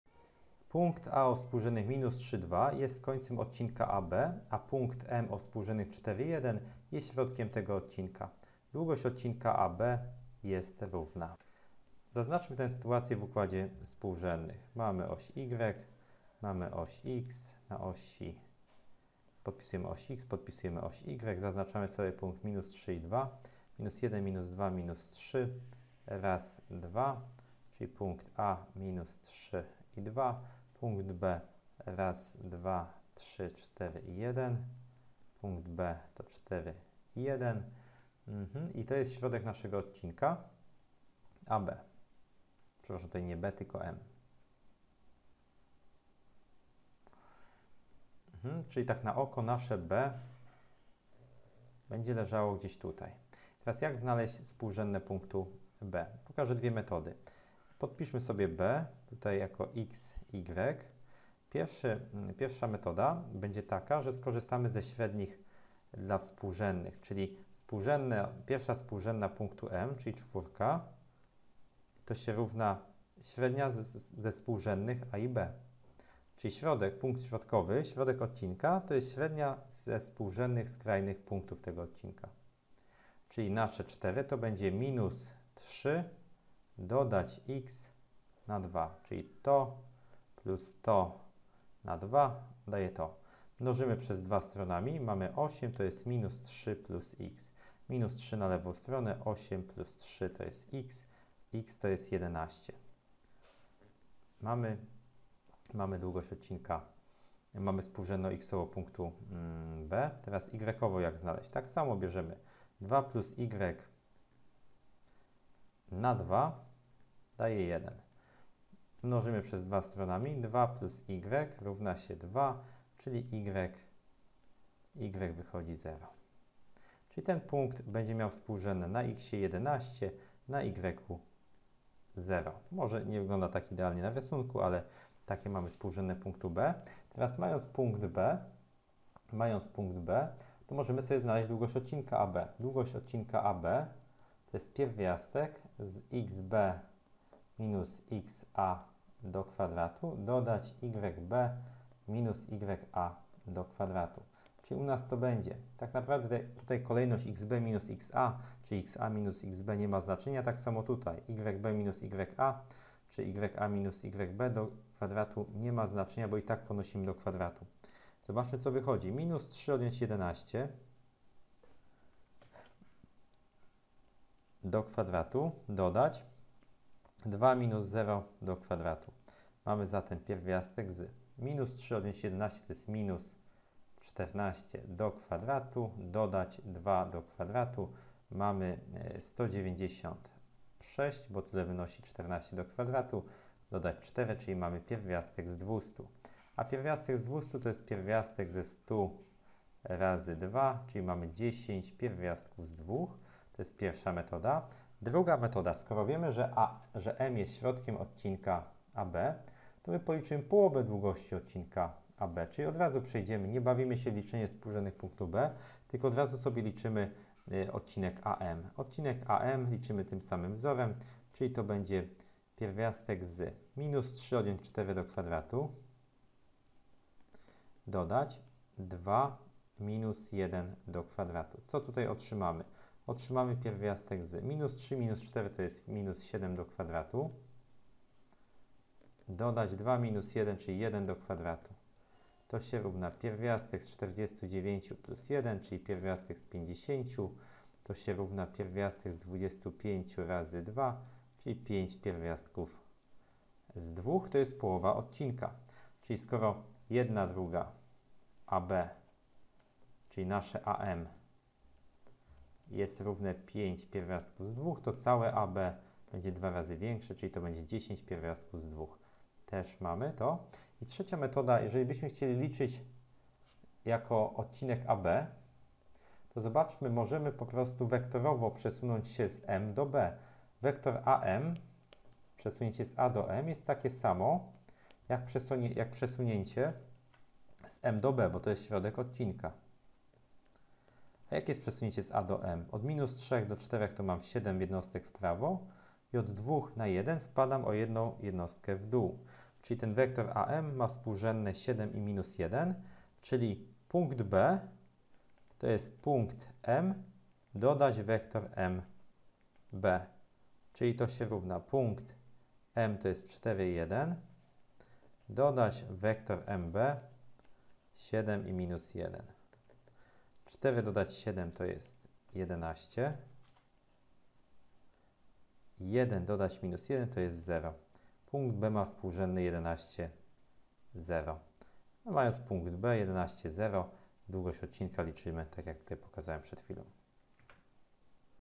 21 Punkt A=−3,2 jest końcem odcinka AB, a punkt M=4,1 jest środkiem tego odcinka Długość odcinka AB
Punkt A=(−3,2) jest końcem odcinka AB, a punkt M=(4,1) jest środkiem tego odcinka. Długość odcinka AB jest równa Źródło